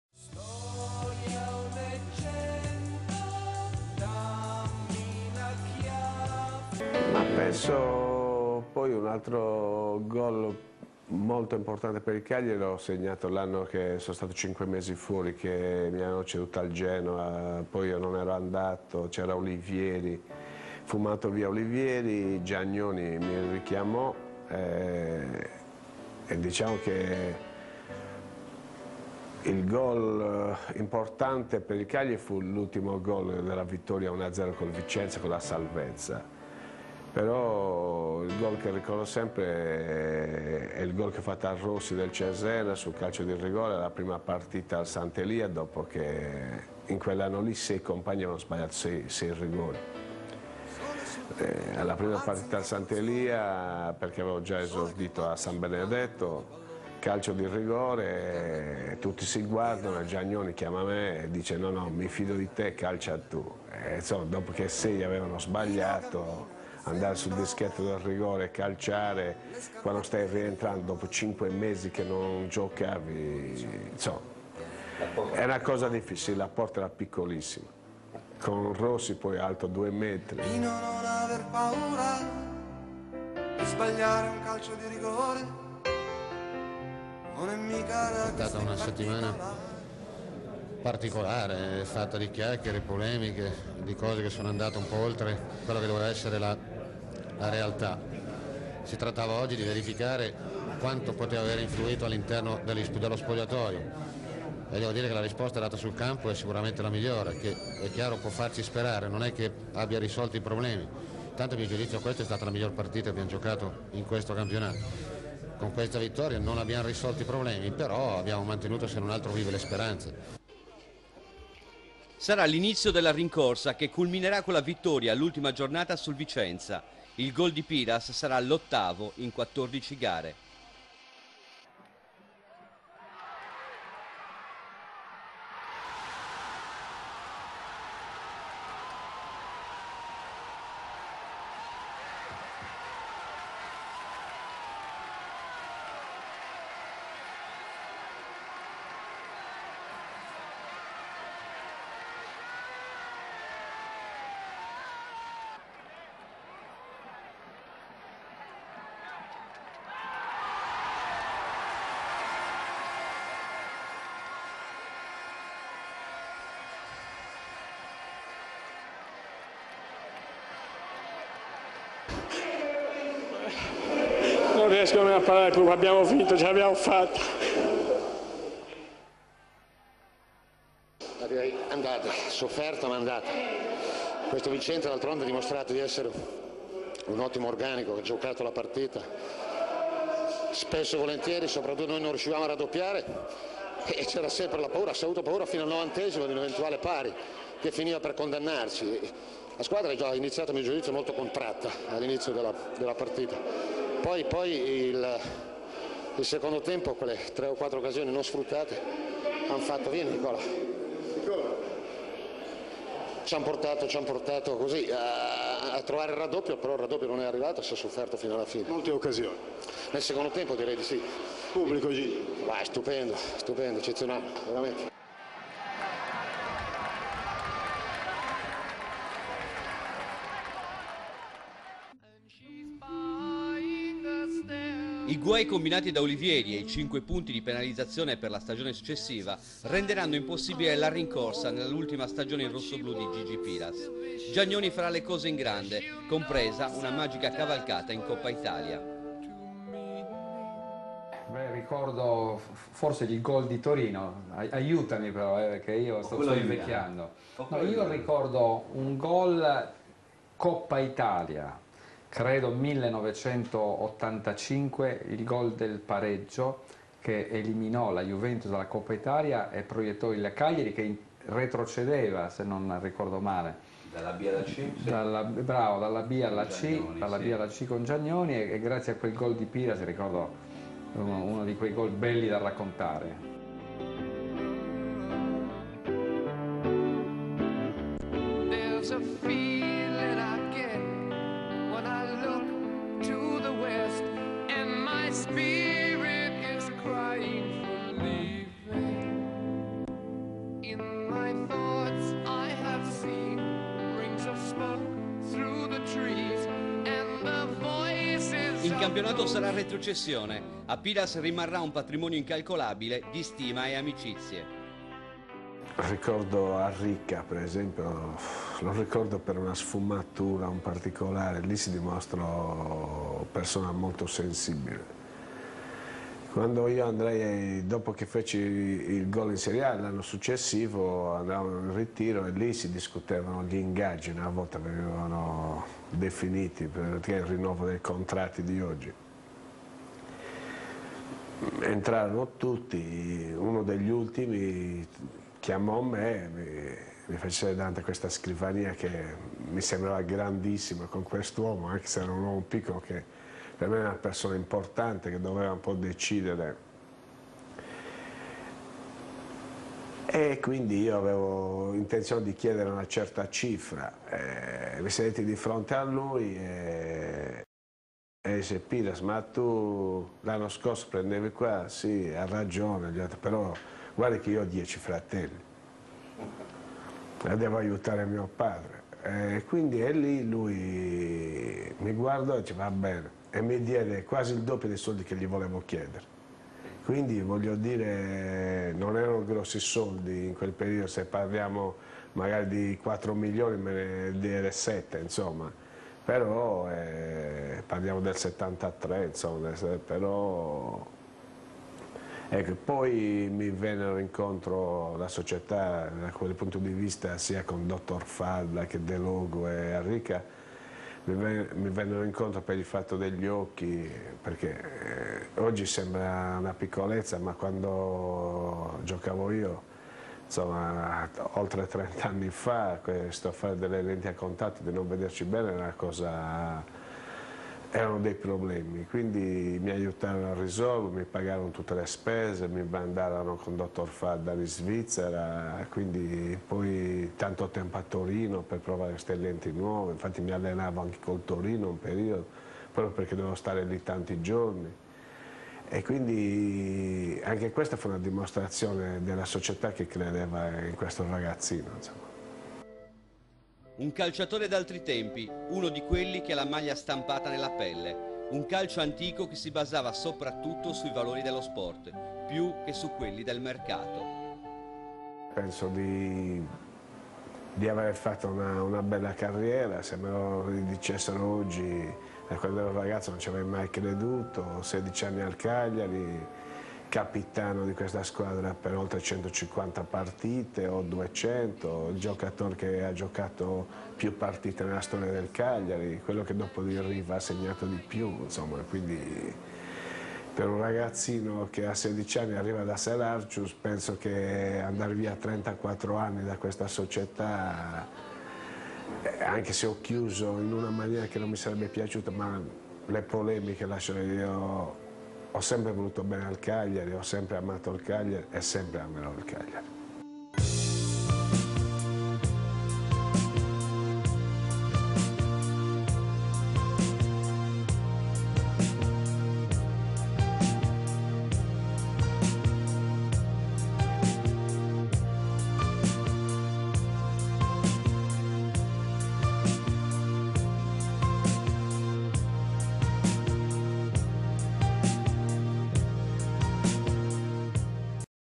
Storie di Sport - Gigi Piras (parte 9/9)
La riabilitazione di Piras è lenta e dolorosa: il Cagliari, con Ulivieri in panchina, inizialmente lo cede al Genoa ma lui non si presenta. Dopo poco tempo Giagnoni torna al timone dei rossoblù e richiama l'attaccante di Selargius. Il suo rientro è dolce: il tecnico chiede a lui di battere un rigore contro il Cesena davanti al suo pubblico, dopo che in quell'anno 6 compagni avevano sbagliato 6 rigori. La porta è piccola, Rossi è un gigante, ma la palla entra. Quegli anni però non saranno ricchi di soddisfazioni: qualche altro gol, gli ultimi della carriera di Piras, che accompagnerà il Cagliari in Serie C e si congederà dal calcio dopo aver portato in C il La Palma.
Terzo cannoniere con la maglia rossoblù, attaccante tra i più felini del calcio italiano, ma soprattutto beniamino della tifoseria cagliaritana che ancora oggi lo ricorda con affetto e benevolenza.
Signore e signori, Gigi Piras.